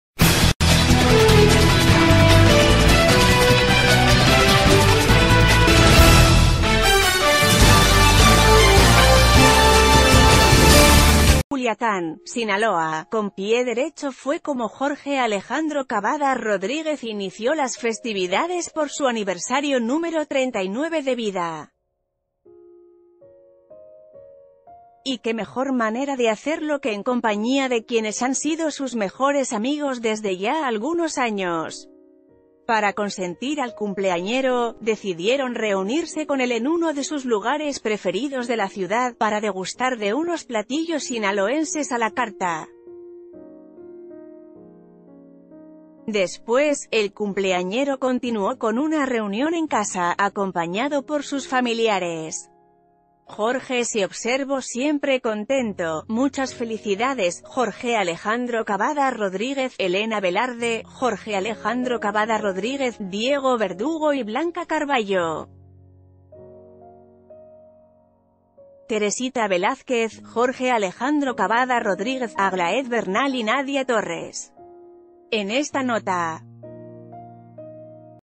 Jorge Alejandro, de cumpleaños
Culiacán, Sinaloa.- Con pie derecho fue como Jorge Alejandro Cabada Rodríguez inició las festividades por su aniversario número 39 de vida. Y qué mejor manera de hacerlo que en compañía de quienes han sido sus mejores amigos desde ya algunos años. Para consentir al cumpleañero, decidieron reunirse c...